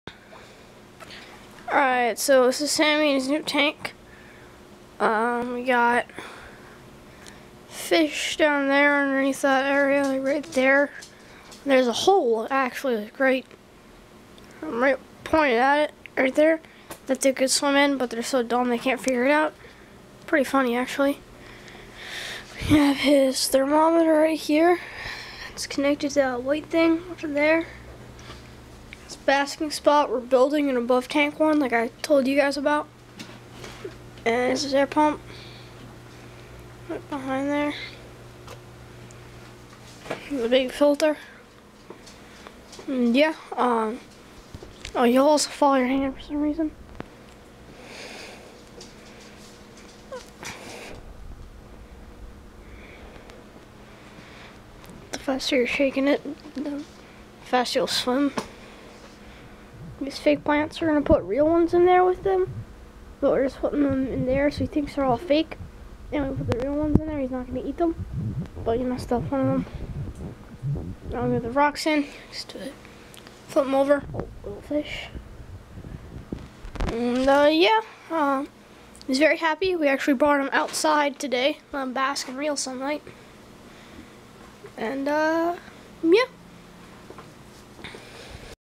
Sammy The Turtle's new home
Owen explains Sammy's new home. This is a 55 gallon aquarium with a Rena XP3 Filter and a Zoomed Turtle Dock. The stand is an Ikea Hemnes TV Bench that has been reinforced to handle the extra weight. Next project is building an above tank basking area so Sammy has more room to roam.